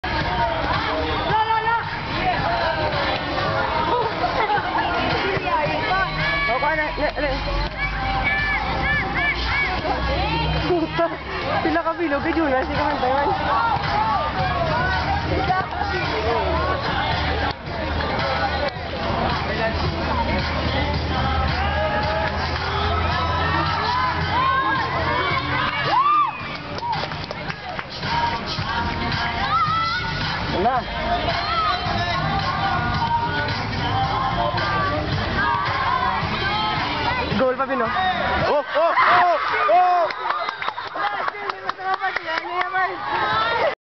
angelicum school iloilo vs ateneo de iloilo
under 12 football festival, angelicum won, 2-1